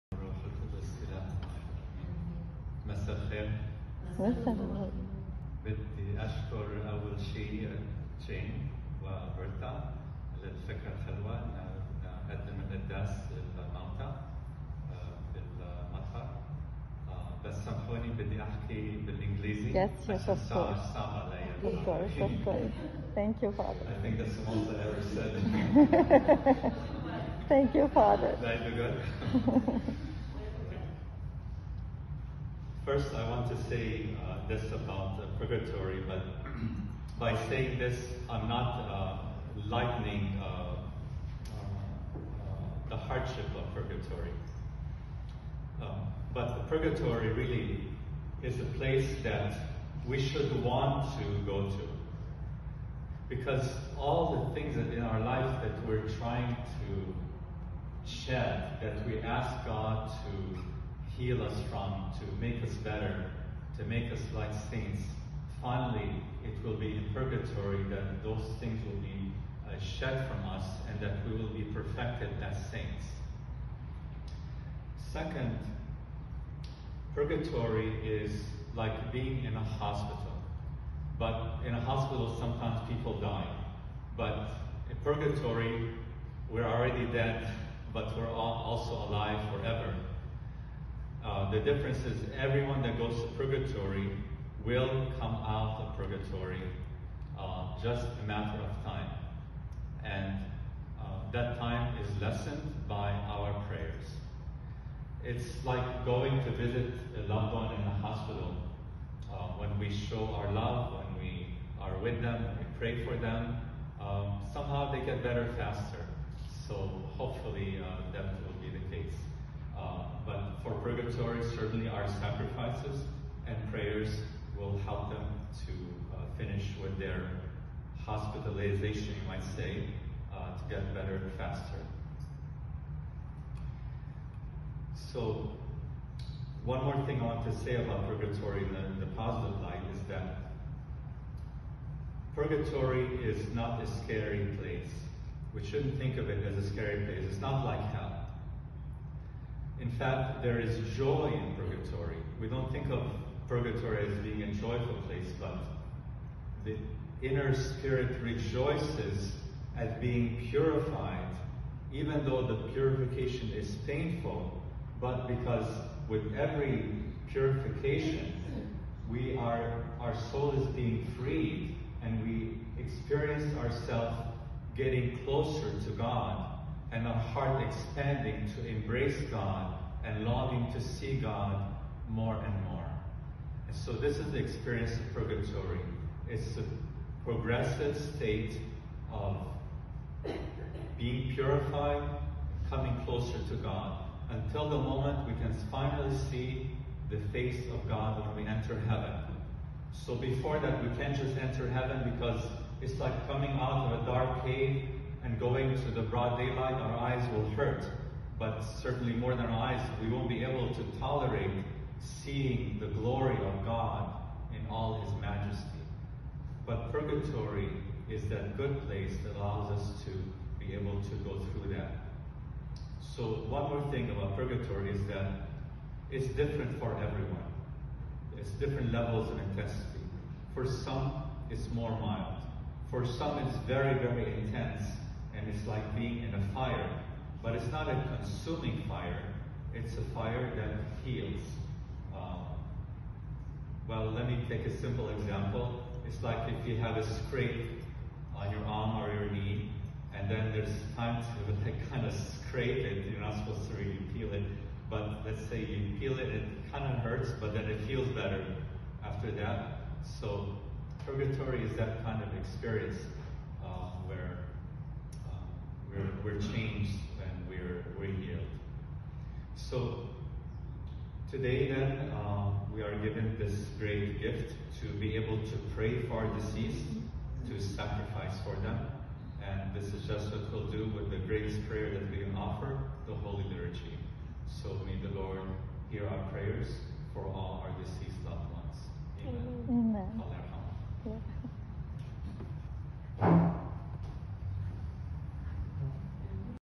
Sermon, "Remember me in Your Kingdom" mass at Saint John Maron Church, Orange, CA.
The Divine Liturgy for our departed in the Hope of Resurrection at Saint John Maron Church, Orange, CA, celebrated by Reverend Monsignor Antoine Bakh.